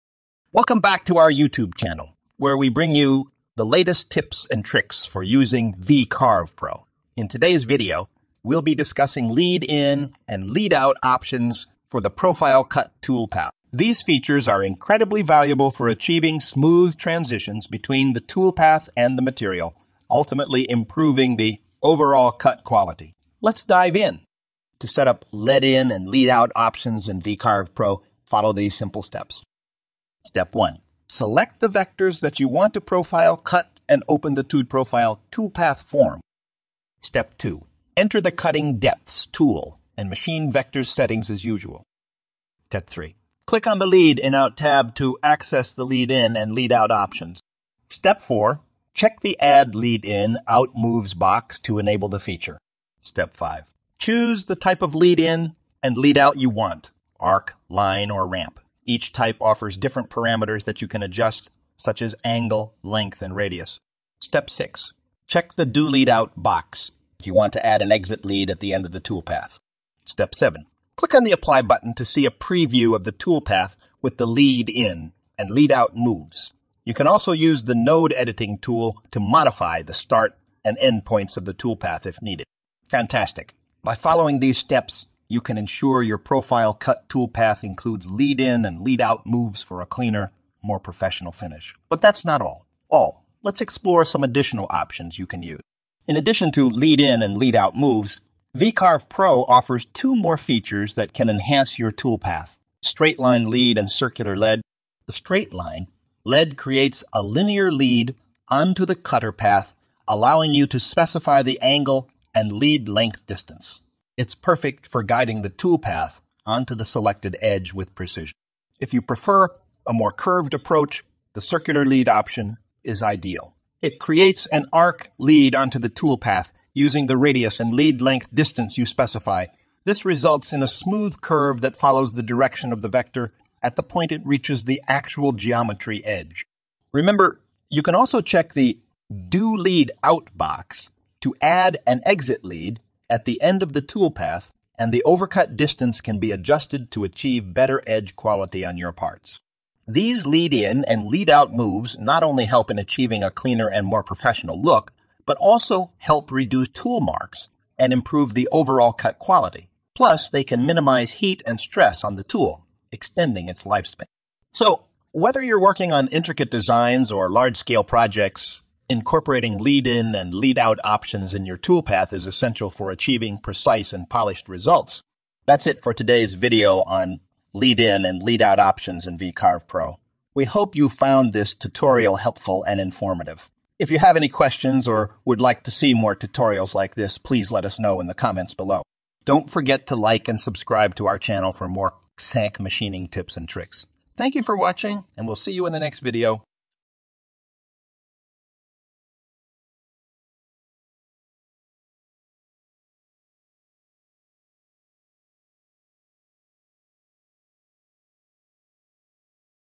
Enhancing CNC Toolpaths: Mastering Lead In and Lead Out Options in VCarve Pro
In this video, we dive into the world of CNC machining and explore how to take your toolpaths to the next level with lead in and lead out options in VCarve Pro. Join us as we guide you through the step-by-step process of setting up these features to achieve smooth transitions and impeccable cut quality. Learn how to reduce tool marks, improve edge quality, and prolong the lifespan of your tools. Discover the power of straight line and circular leads, and unlock the potential for precision and professionalism in your CNC projects. Don't miss out on this essential tutorial for enhancing your CNC toolpaths. Watch now and elevate your machining skills to new heights!